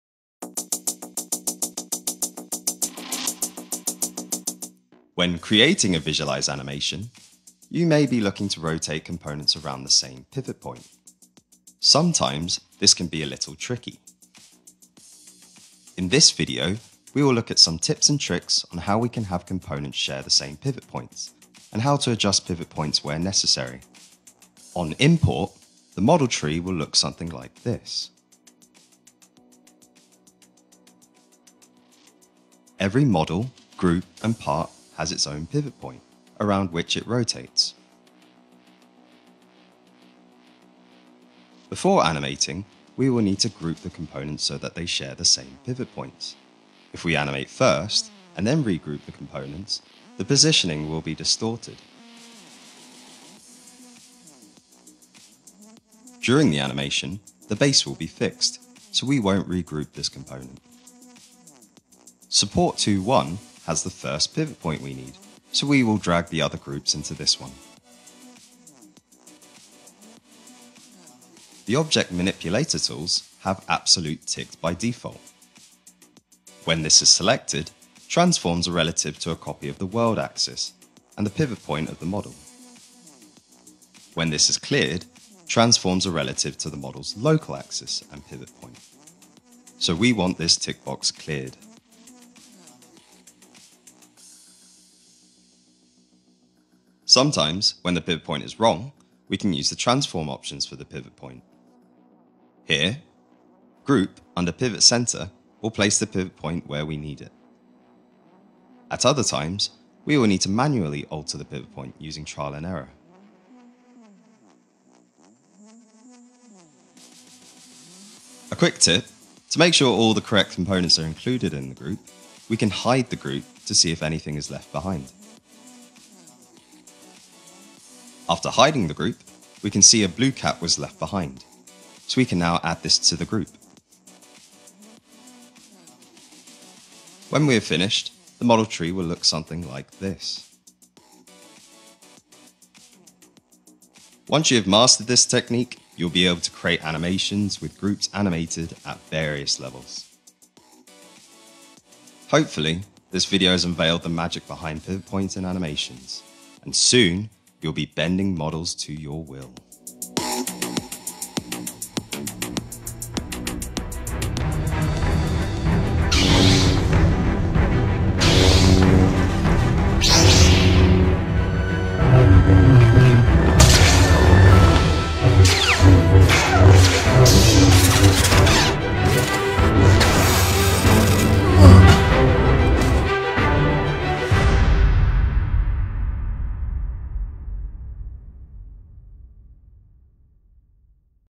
How to Animate Pivot Points in SOLIDWORKS Visualize | Beginner SOLIDWORKS Tutorial
Learn how to utilise pivot points in SOLIDWORKS Visualize to enhance your animations with this quick tutorial.